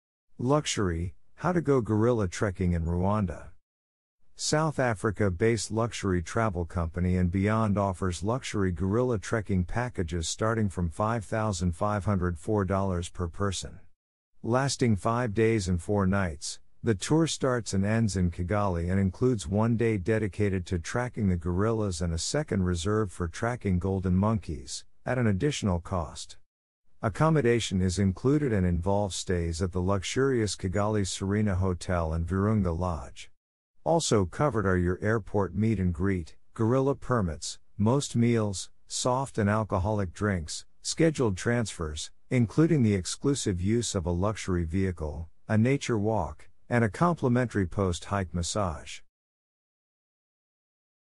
Luxury
Luxury - How to Go Gorilla Trekking in Rwanda.
South Africa-based luxury travel company andBeyond offers luxury gorilla trekking packages starting from $5,504 per person. Lasting five days and four nights, the tour starts and ends in Kigali and includes one day dedicated to tracking the gorillas and a second reserved for tracking golden monkeys (at an additional cost). Accommodation is included and involves stays at the luxurious Kigali Serena Hotel and Virunga Lodge. Also covered are your airport meet-and-greet, gorilla permits, most meals, soft and alcoholic drinks, scheduled transfers (including the exclusive use of a luxury vehicle), a nature walk, and a complimentary post-hike massage. .